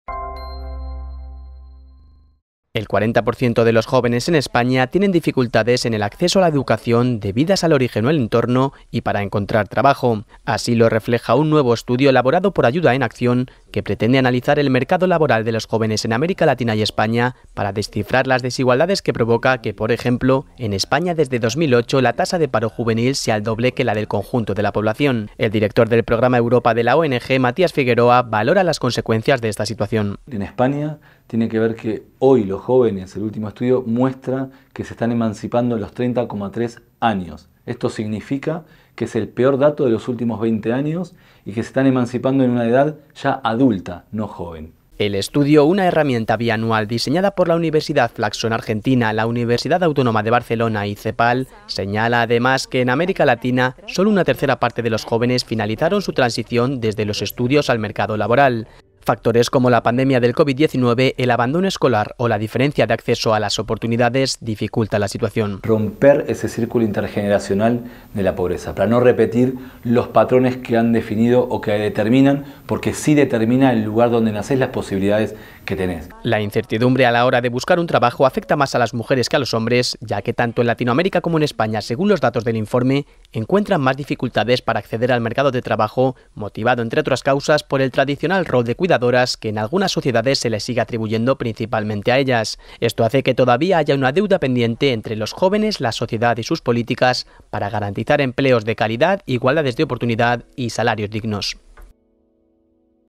Los jóvenes iberoamericanos encuentran desventajas en el acceso laboral, según un informe
Madrid, 30 oct (EFE).- Los jóvenes en España y América Latina hallan desventajas en el acceso laboral, donde casi la mitad de la juventud española tiene dificultades y más del 50 % de las mujeres jóvenes latinoamericanas no habían accedido al empleo al inicio del 2020, según publicó hoy un estudio de la ONG Ayuda en Acción.
La tendencia hacia la postergación de la inserción laboral en aquellos que no empezaron la transición, tanto en América Latina como en España, se incrementó en un 10 puntos porcentuales respecto al anterior estudio.
Para deducirlo se ha valido del Índice de Transiciones en Desventaja (ITed), una herramienta bianual diseñada por la Universidad FLACSO en Argentina, la CEPAL y la Universidad Autónoma de Barcelona, que pretende conocer las desigualdades educativas, laborales y de género detrás de los datos de desempleo. EFE